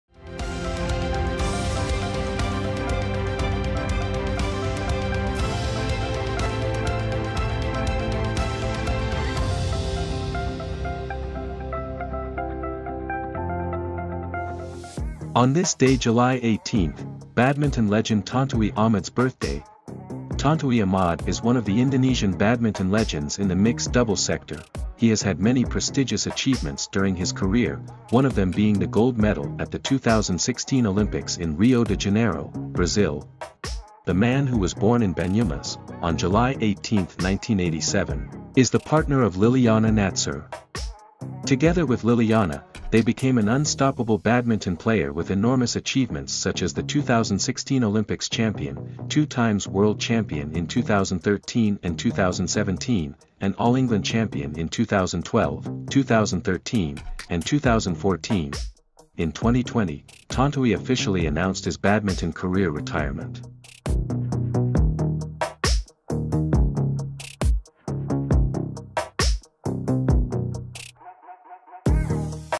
On This Day July 18th: Badminton Legend Tontowi Ahmad's Birthday
Tontowi Ahmad is one of the Indonesian badminton legends in the mix double sector.

He has had many prestigious achievements during his career, one of them being the gold medal at the 2016 Olympics in Rio de Janeiro, Brazil. See more in the video.

Script / Video Editor: Aulia Hafisa / Praba Mustika